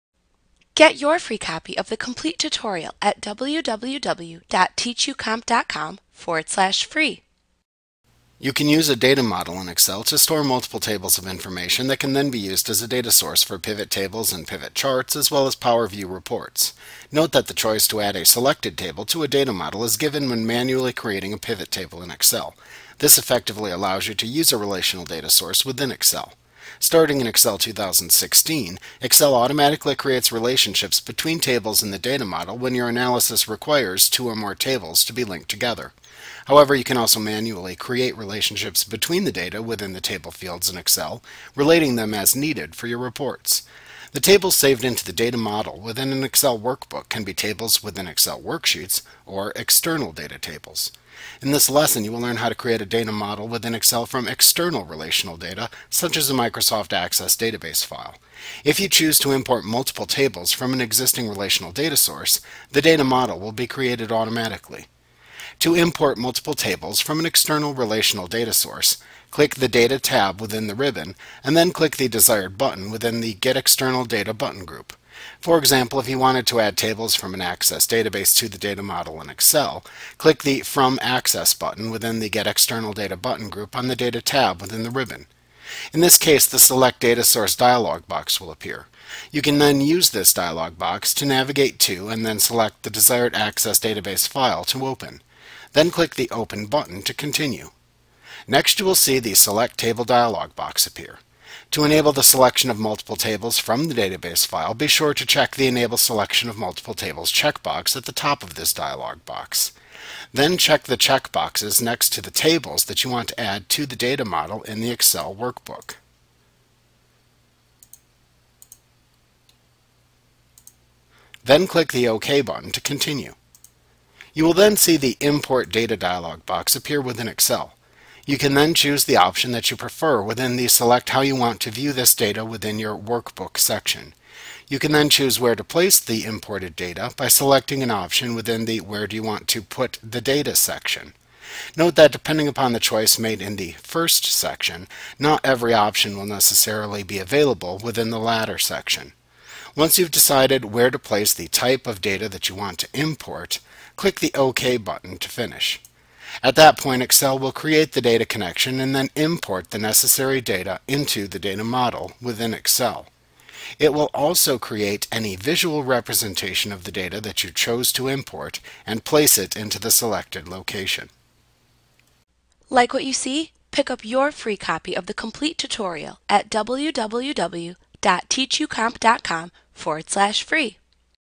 Excel 2016 Tutorial Creating a Data Model from External Relational Data Microsoft Training Lesson
Learn about Creating a Data Model from External Relational Data in Microsoft Excel A clip from Mastering Excel Made Easy v. 2016. - the most comprehensive Excel tutorial available. Visit us today!